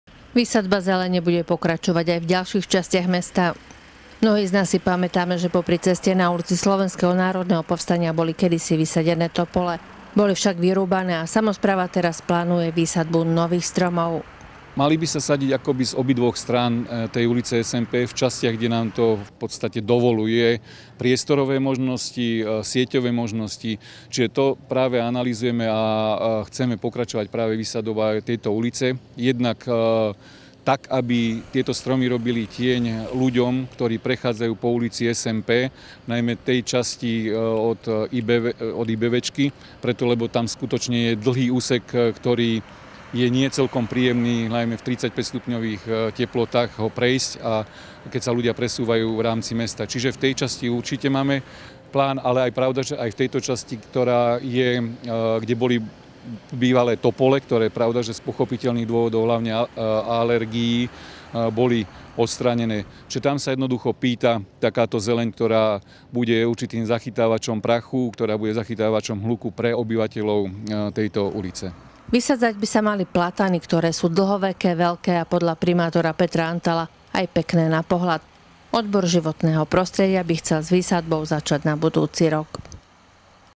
Nové stromy nahradia aj vypílané topole
Výsadba zelene bude pokračovať aj v ďalších častiach mesta.